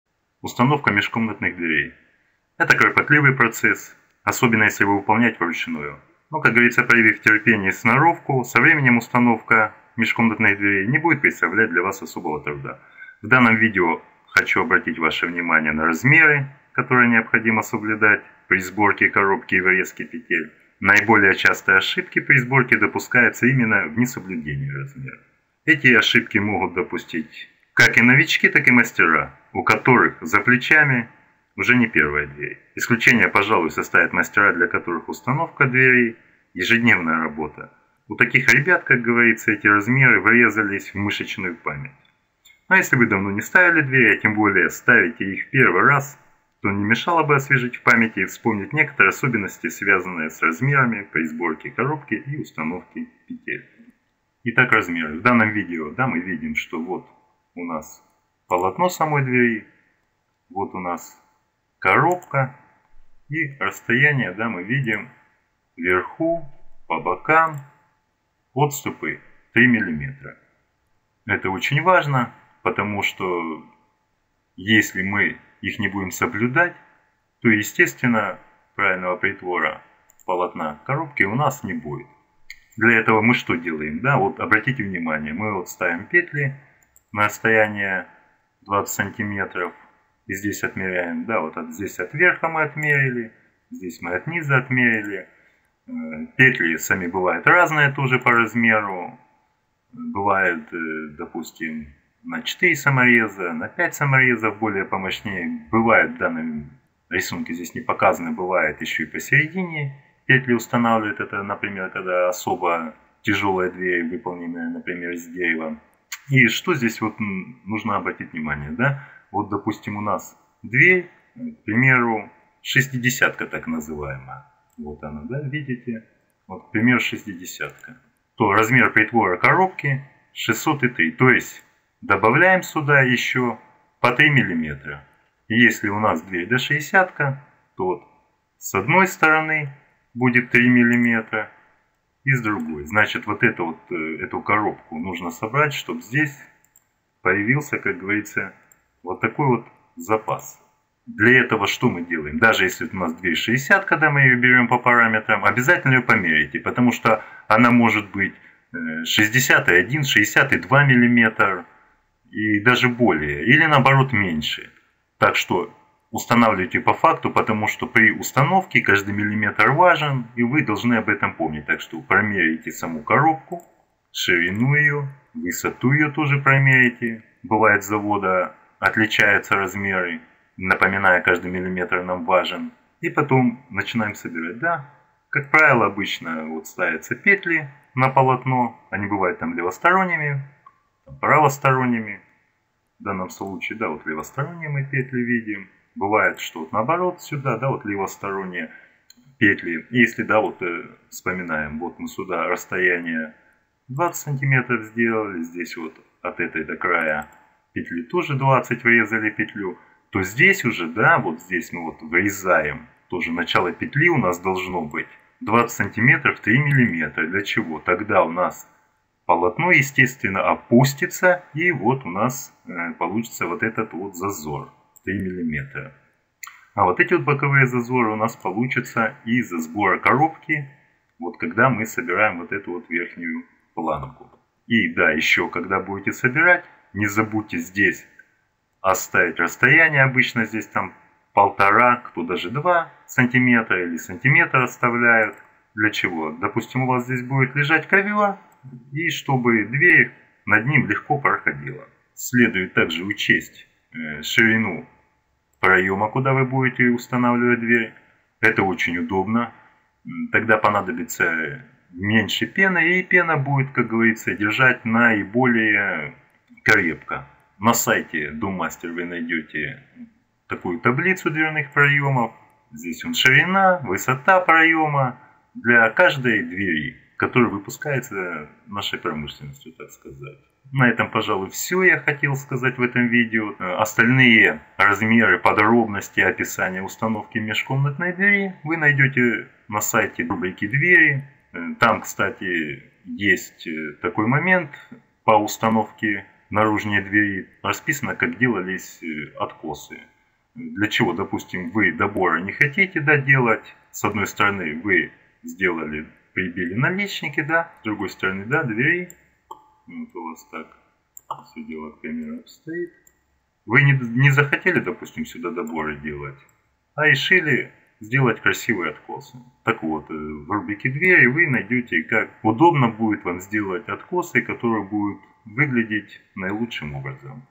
Установка межкомнатных дверей - Сборка - Таблица размеров
Установка межкомнатных дверей своими руками. Таблица дверных проемов. Установка петель и замка. Сборка дверной коробки. Телеграмм канала